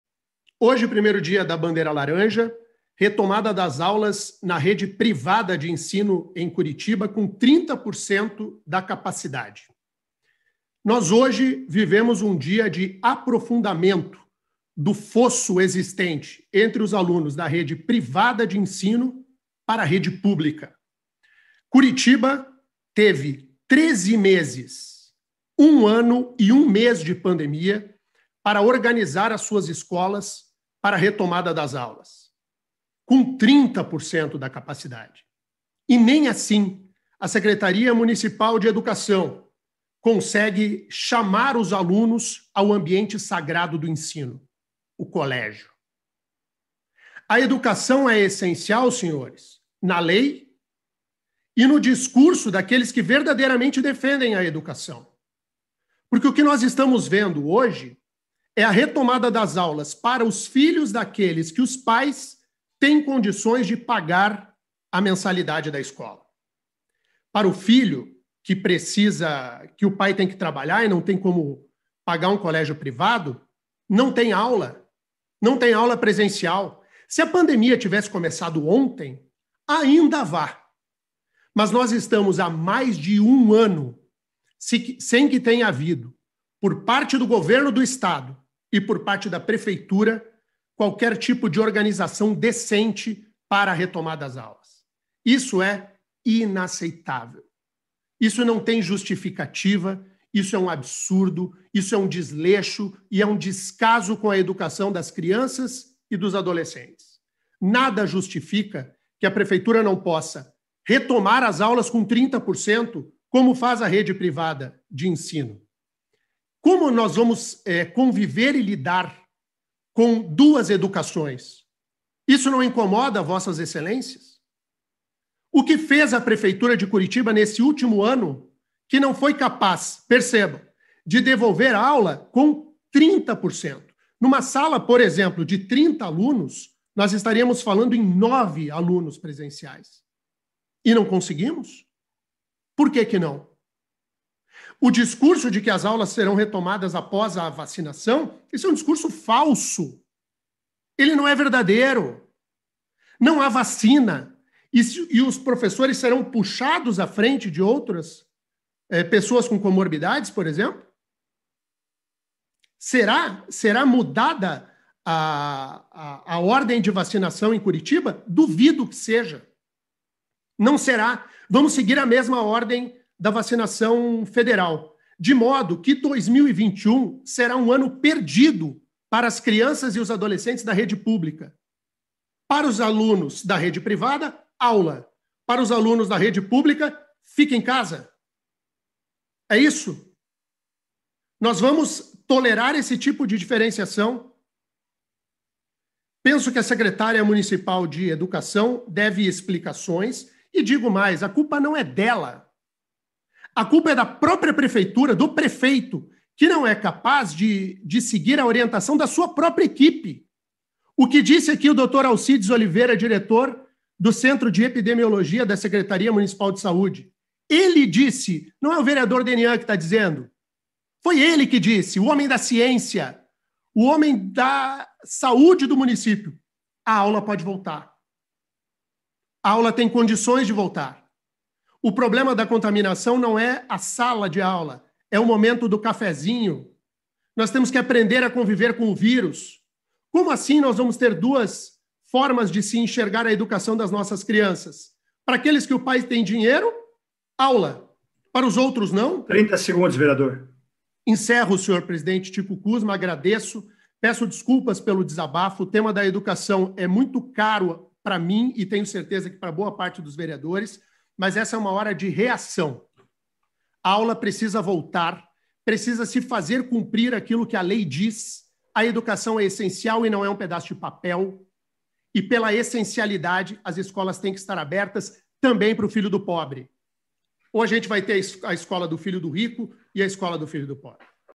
Na Câmara de Curitiba, Denian Couto cobra retorno das aulas presenciais na rede municipal
Em sessão plenária virtual da Câmara Municipal de Curitiba, falei sobre o retorno das aulas presenciais nas redes pública e privada da capital paranaense.

Em 13 meses de pandemia e os governos do Paraná e de Curitiba não conseguiram criar uma estrutura mínima para receber 30% dos alunos. Rede privada voltou hoje. O fosso da desigualdade só aumenta. Educação, para eles, só é essencial no discurso. Educação é essencial, mas descaso com o que é prioridade segue a pleno.